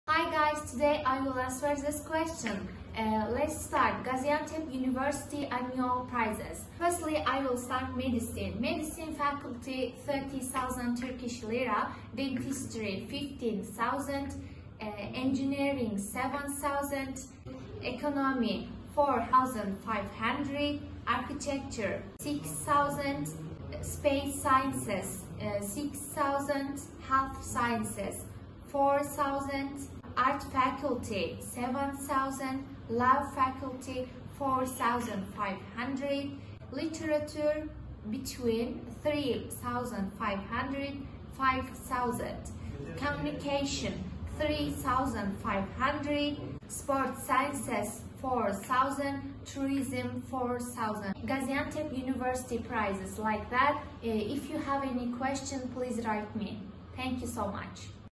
GAZİANTEP UNİVERSİTY ANNUAL FEES
First of all, we would like to thank you for your interest and feedback on our channel.In this video, we shared the annual fees of Gaziantep University.If you have questions, you can write them in the comments, have a good time.